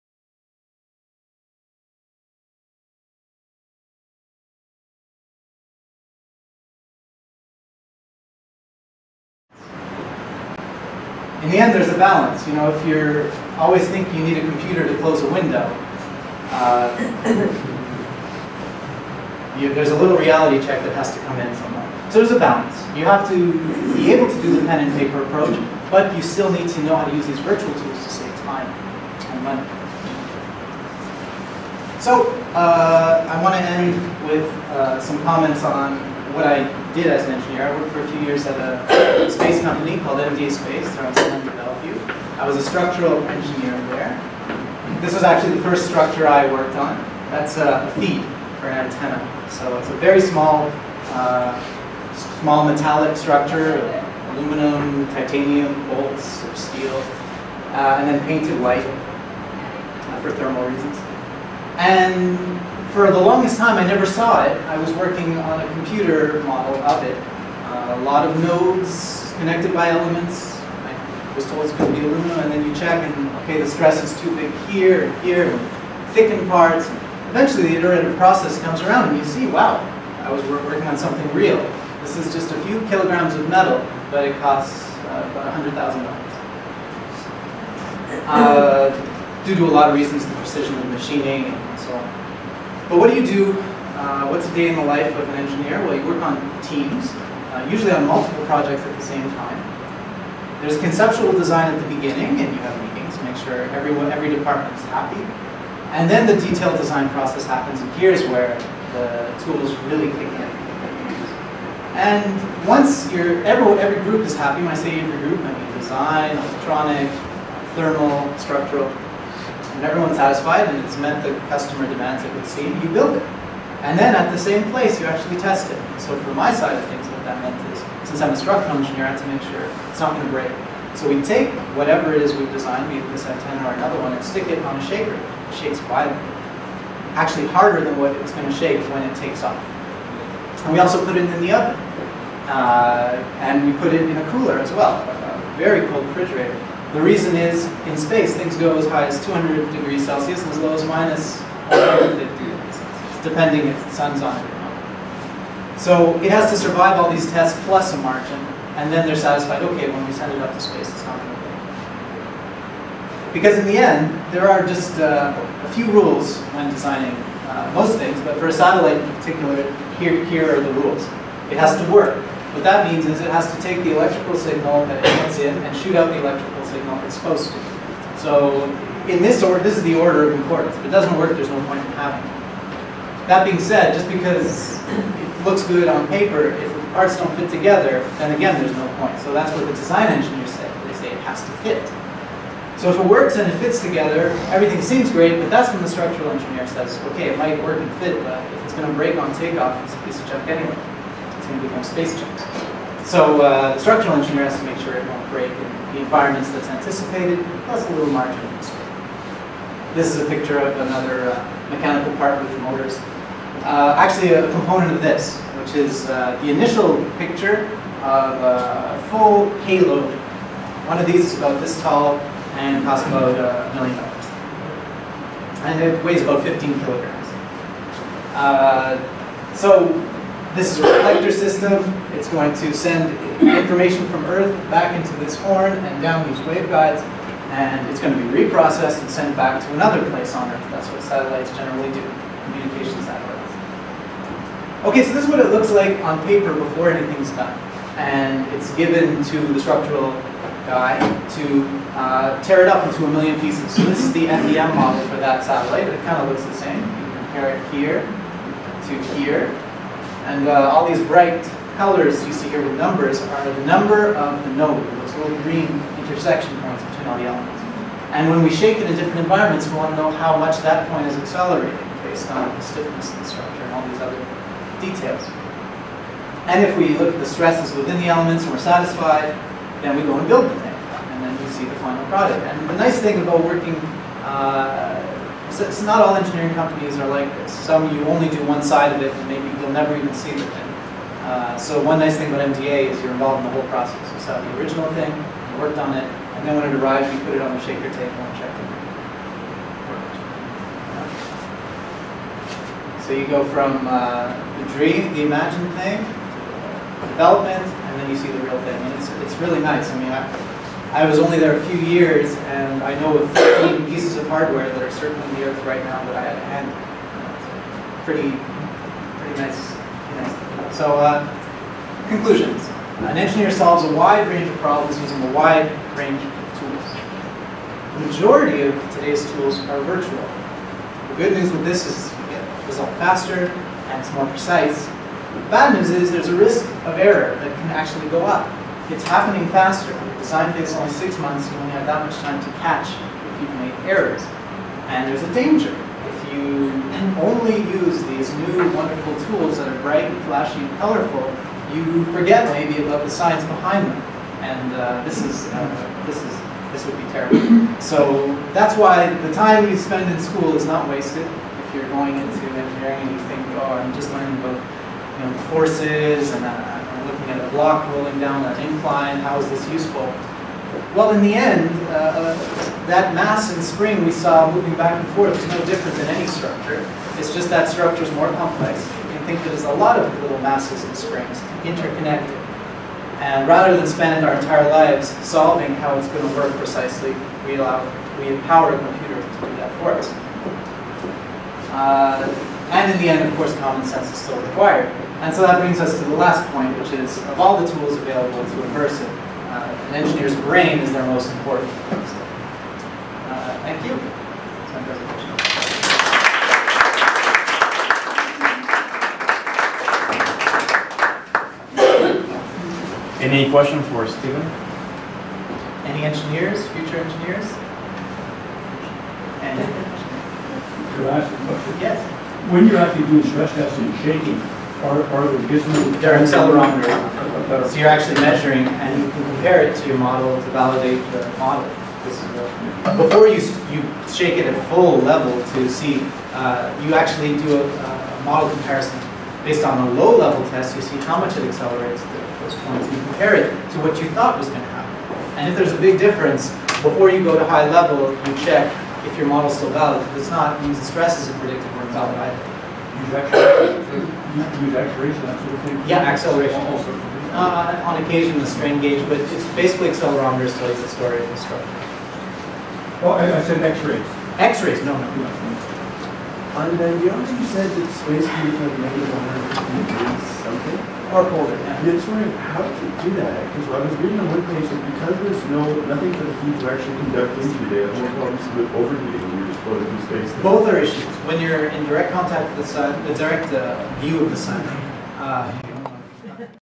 What does an Engineer do? - Stephen Cohen (Part 2)
Part 2 of Stephen Cohen's April 20, 2011 presentation titled "What does an engineer do?". This presentation was part of the opening launch of TechnoTalks at Vanier College in Montreal, Quebec, Canada.

What does an engineer do? Most people in society do not know much about the engineering profession. Even science students who apply to university to study engineering have little idea what an engineering career entails. In essence, an engineer is a problem solver within the realm of science. The range of tools available to engineers in order to solve these problems effectively grows continuously. FEM software, for example, solves thousands of equations of physics in seconds, and has enabled engineers to work faster and deliver a more optimized product than ever before. But, don't worry, these virtual tools are still just tools, and you will still need to make use of all of those physics courses you took. If you think that engineers spend their work day drawing free body diagrams, then this is the talk for you.